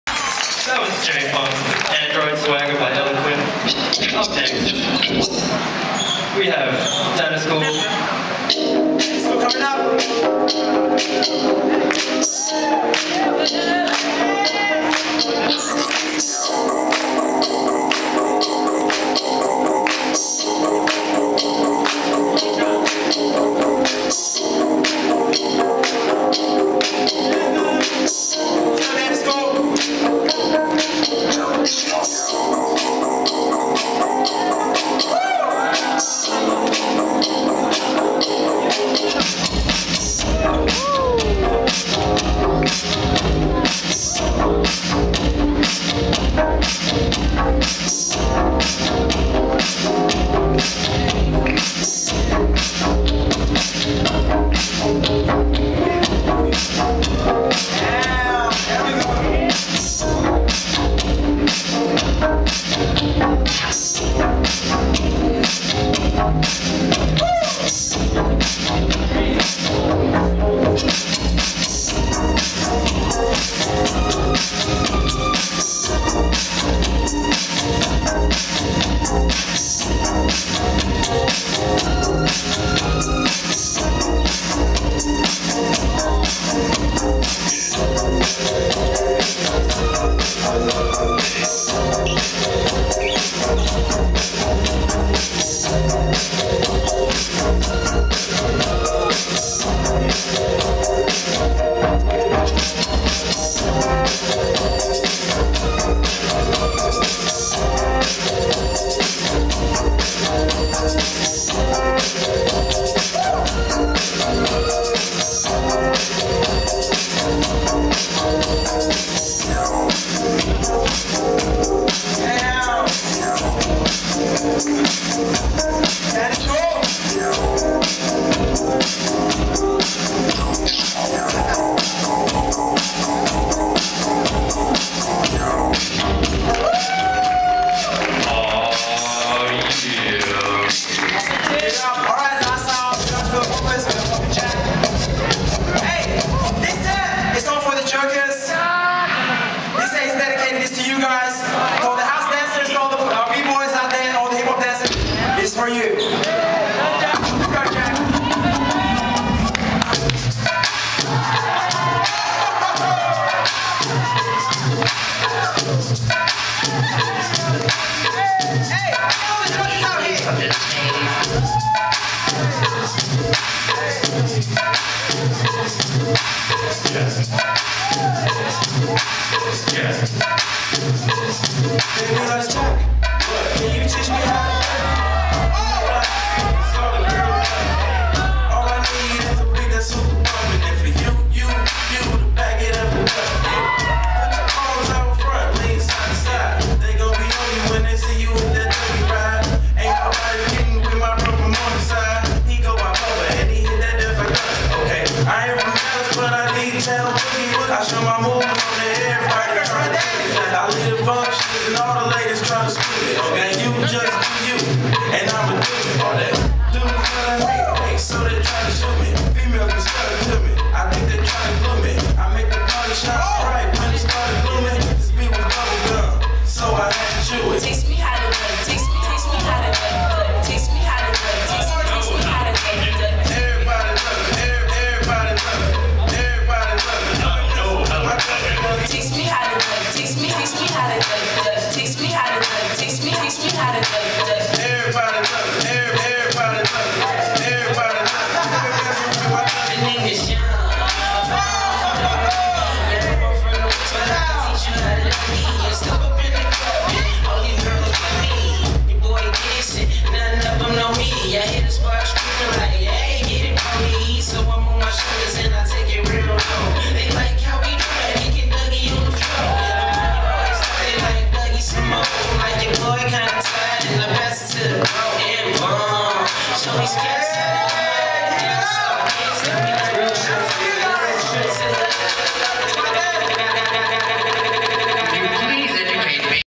Danis Kool & Poppin Jack (Popping Judge Showcase)
Popping Judges Showcase (Prodigy Street Dance Battle) @ UNSW Round House (Sat 22nd Jan 2011)

Danis Kool (HFz) & Poppin Jack (SBz & 603)